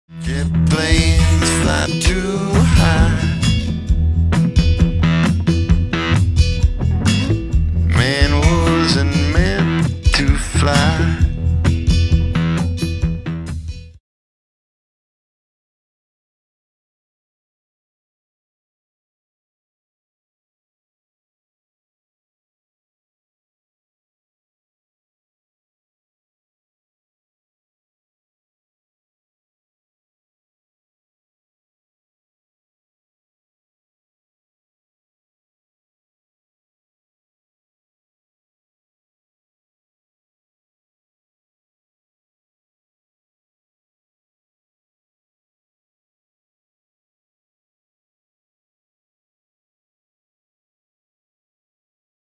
The Blend Surfboard By Formula Energy
Formula Energy presents the Blend model surfboard . Watch our team rider tearing the bag out of it !